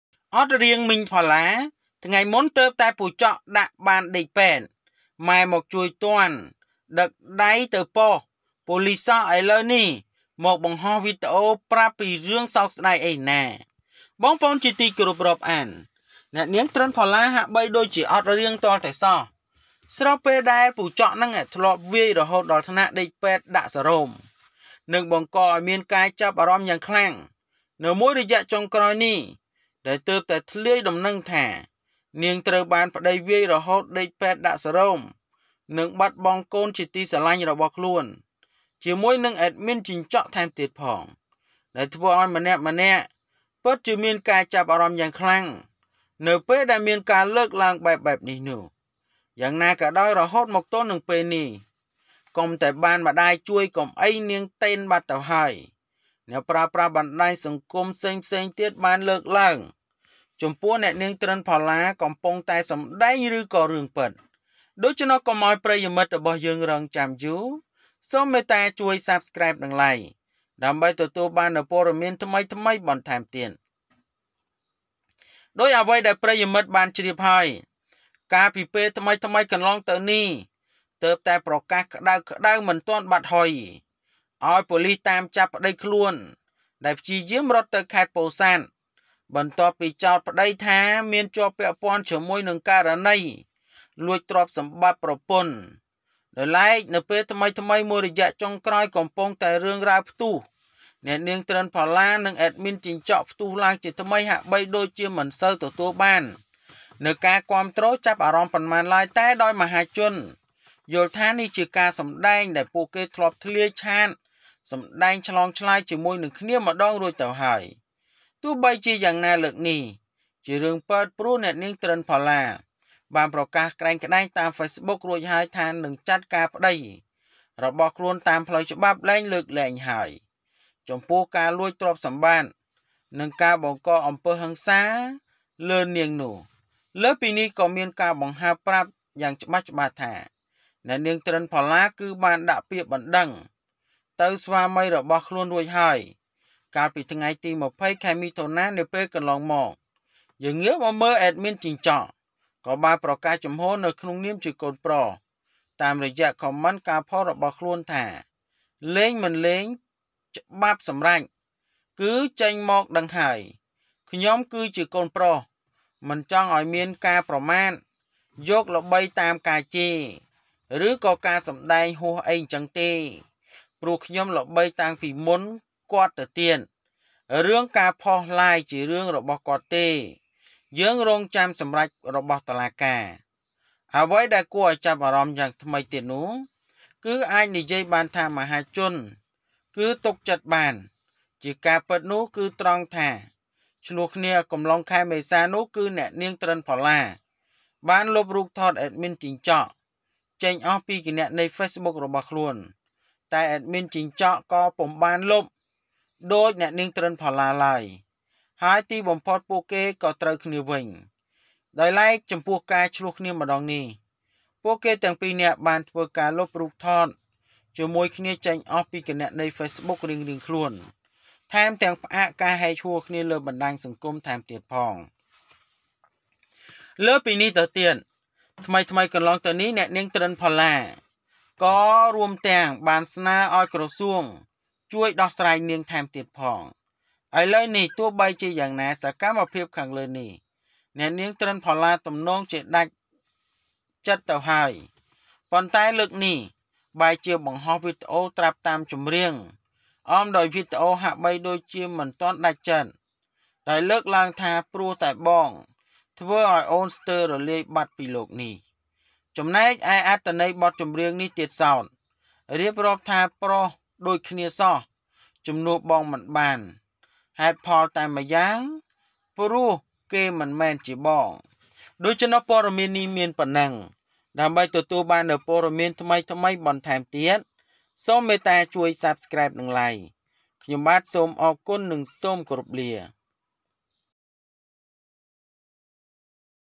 ថ្ងៃមុនទើបតែពូចក់ ដាក់ដេកពេទ្យ បានម៉ែមកជួយទាន់ ដឹកដៃទៅប៉ុស្ដិ៍ប៉ូលីសសោះ អីលូវមក បង្ហោះវីដេអូហើយ
ថ្ងៃមុនទើបតែពូចក់ ដាក់ដេកពេទ្យ បានម៉ែមកជួយទាន់ ដឹកដៃទៅប៉ុស្ដិ៍ប៉ូលីសសោះ អីលូវមក បង្ហោះ​វីដេអូ​ ហើយ​ប្រាប់​ថា ព្រោះតែបងធ្វើឲ្យអូន The day before, my uncle had just taken me to the hospital, so my mother came to help and took me to the police station.